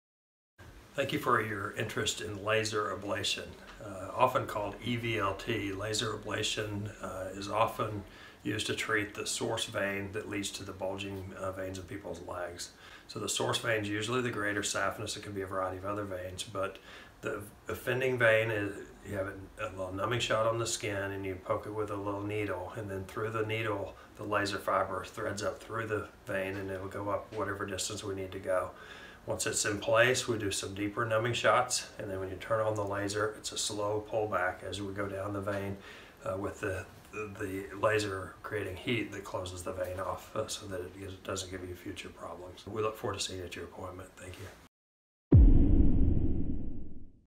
Laser Ablation for Varicose Veins - Dr. Haney | Rogers, Arkansas
Dr. Haney briefly explains laser ablations, one of the most tried and true treatments for varicose veins. Dr. Haney has over 13 years of experience specifically in vein treatment for varicose and spider veins.